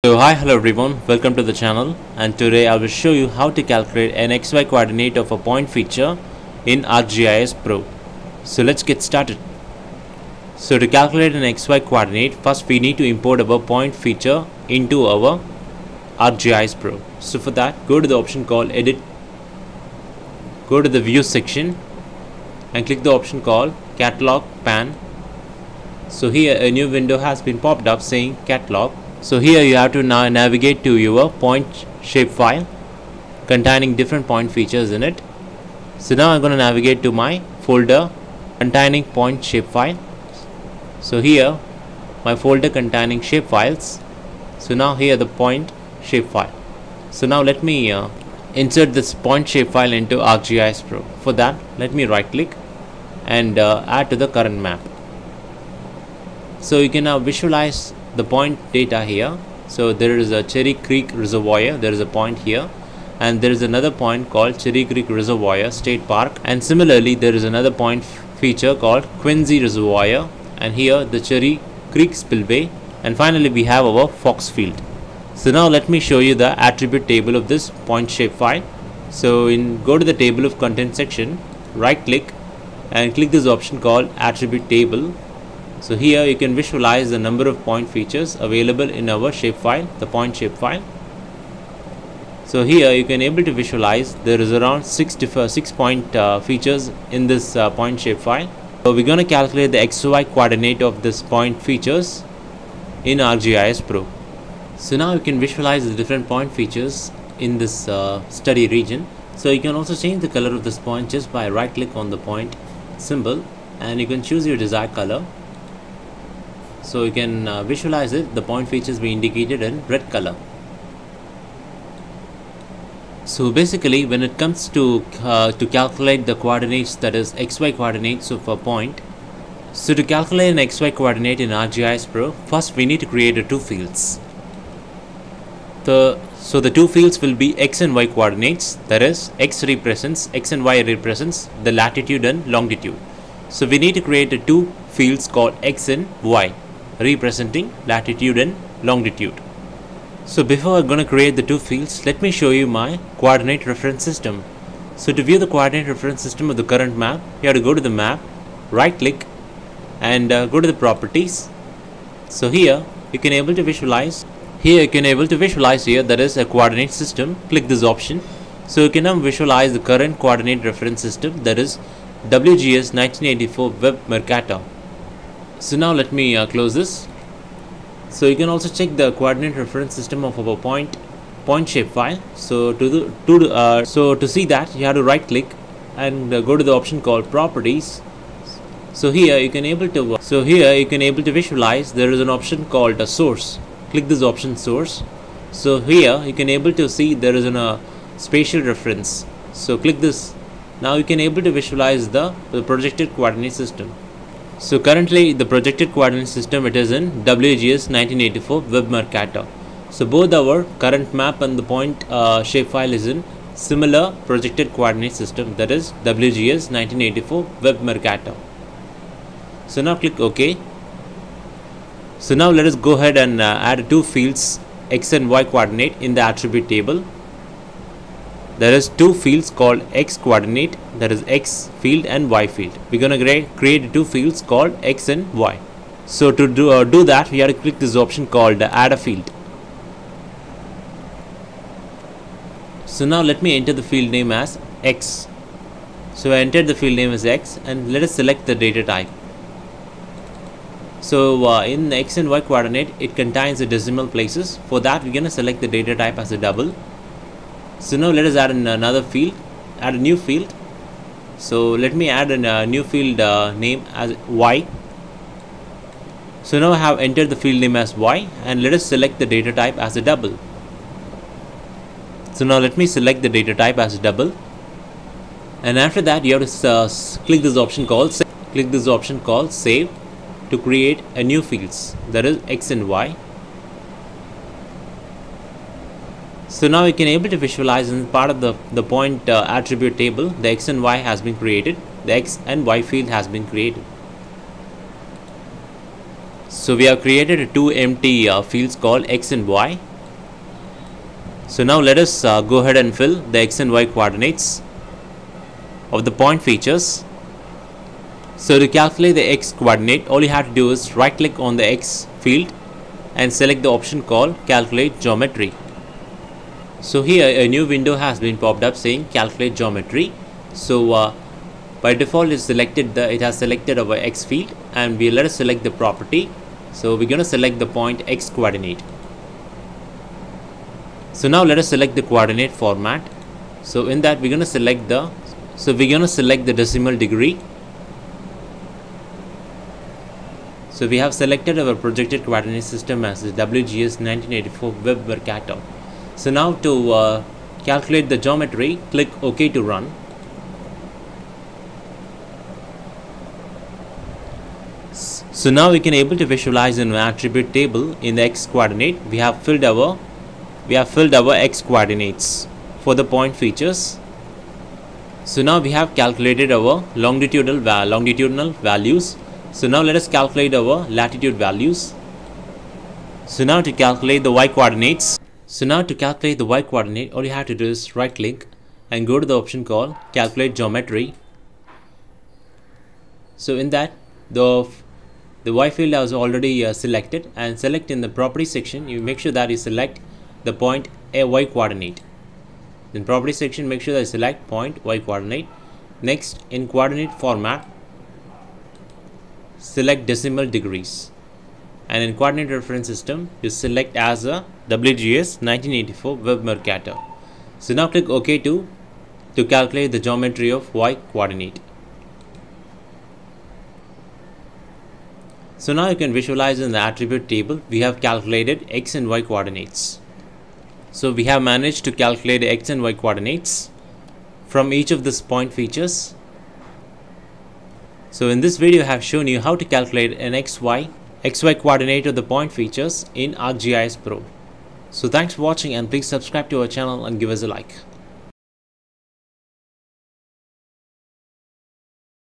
Find X Y Coordinate of Point Feature in ArcGIS Pro
In this tutorial, we will learn how to find the X and Y coordinates of a point feature in ArcGIS Pro. Knowing the exact location of a point features Latitude (X ) and longitude (Y) is crucial for various GIS analyses and map-making tasks. We'll walk you through the step-by-step process, starting from loading your data to extracting the coordinates.